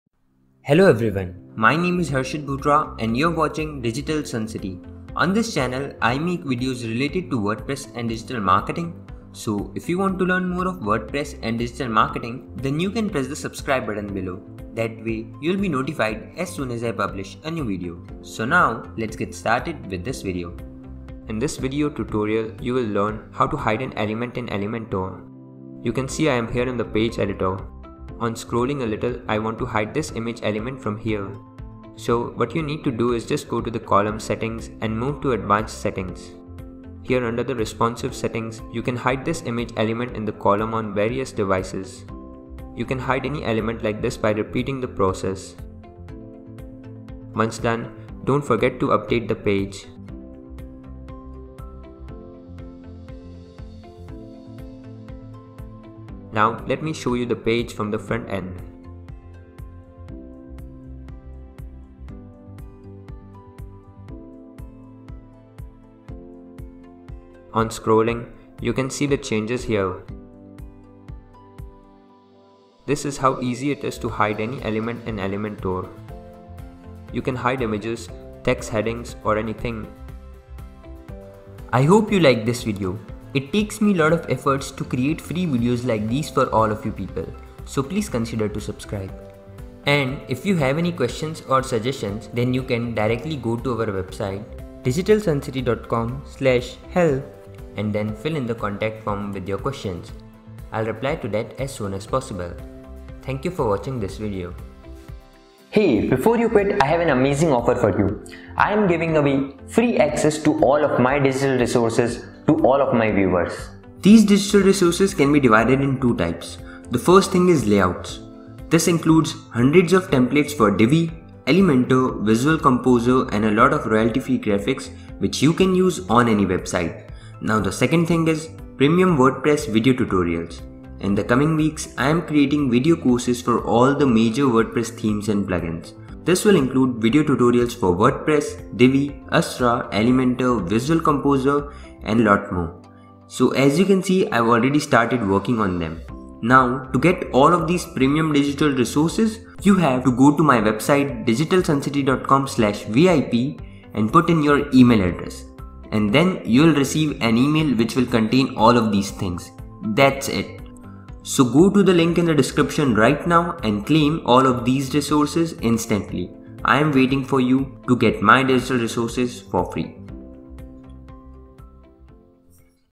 How To Hide An Element In Elementor | WordPress Tutorial
Do you want to hide an element in Elementor?🤔

►In this video, you’ll learn how to hide an element in Elementor. Whether you're a beginner or an advanced user of Elementor, this tutorial will be beneficial for you.

►You'll be going through the step-by-step process of hiding an element in Elementor and providing you with tips and tricks to make the process as easy as possible.

So, sit back, relax,😍and let's get started on hiding elements in Elementor💻